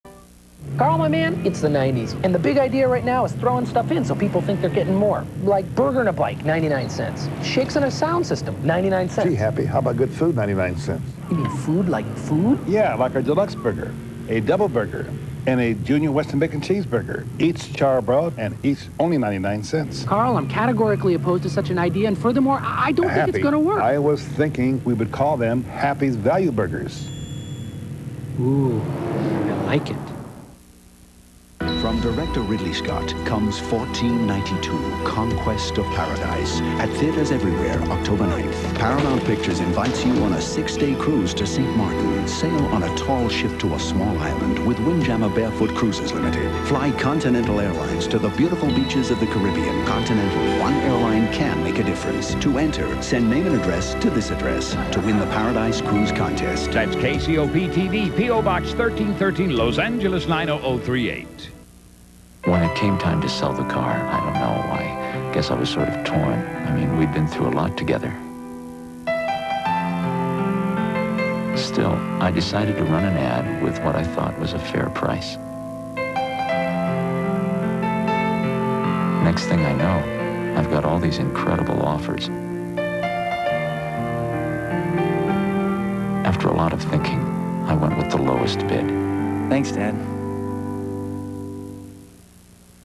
KCOP commercial break (September 28, 1992) Part 5
Recorded off KCOP-TV Channel 13 in Los Angeles, CA

Carl's Jr. Happy Value Burgers
1492: Conquest of Paradise Cruise Contest
Mercedes-Bens Cars